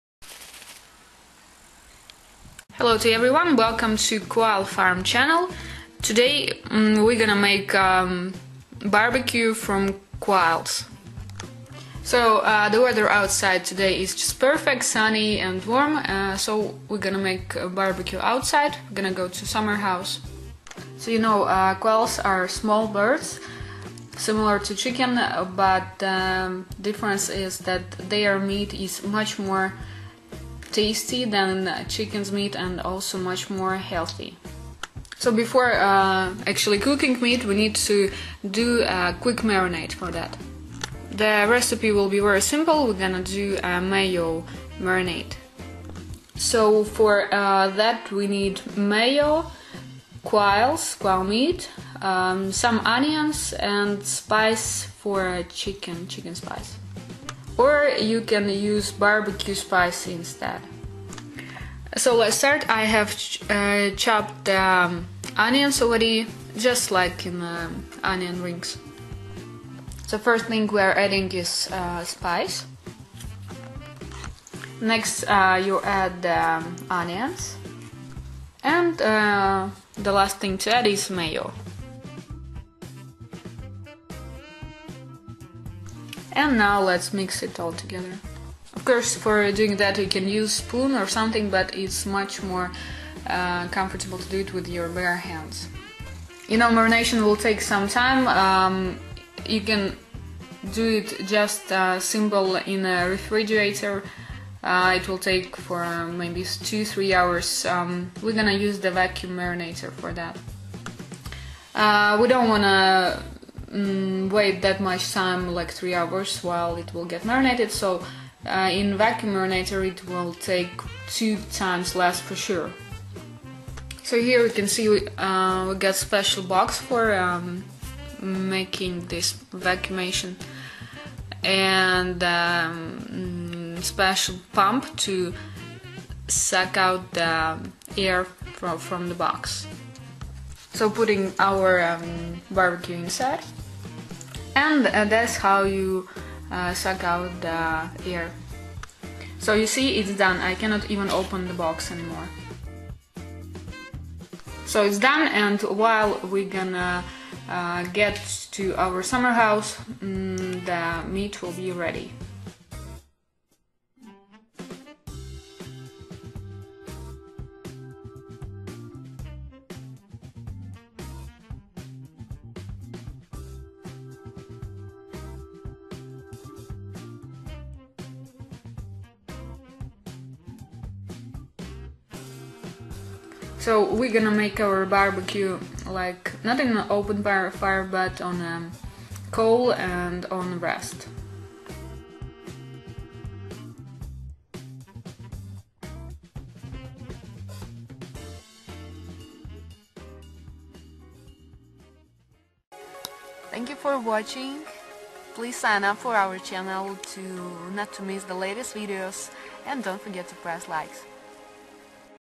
Quails on grill - best recipe
Best option if you feel like eating out, but usual bbq is not enough. Quails' meat is much more healthy and defenitely will make you bbq party taste unusual. Also perfect option if you are expecting guests or having special occasion. Quails' meat is very tender and you don't need too much spices or extraordinary marinate for that - not to spoil the taste.

Ingredients:
Quails 1 kg
Onions 2-3m pcs
 Mayo 3-4 tbsp
Bbq or chicken spice 2 tbsp

food, cooking, meat, bird, bbq, barbecue, marinate, marinated quails, grilled quails, quail meat for sale